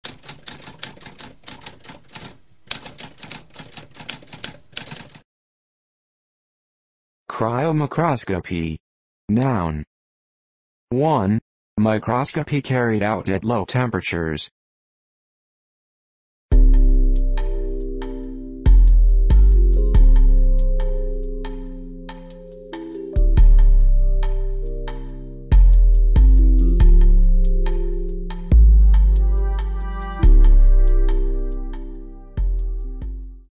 What does cryomicroscopy mean?
A spoken definition of cryomicroscopy.

Intro Sound:
Typewriter - Tamskp